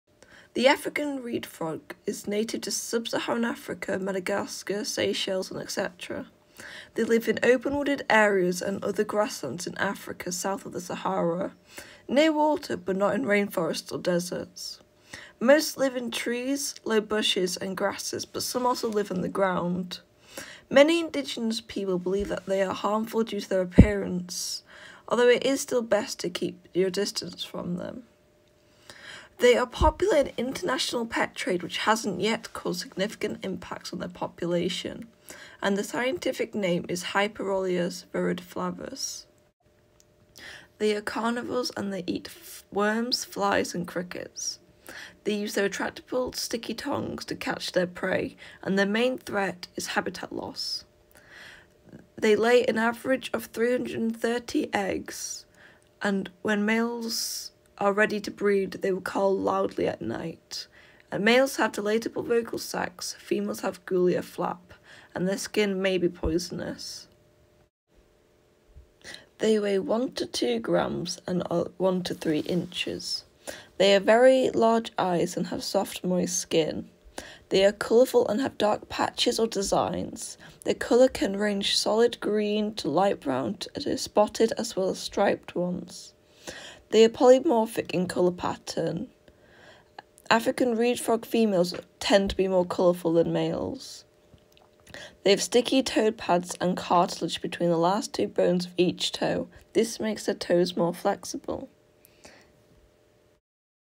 African Reed Frog
I hope everyone enjoyed my video about the African Reed Frog!
The next video will be about the American Spadefoot!

All images were found online and the information was found from the following sources: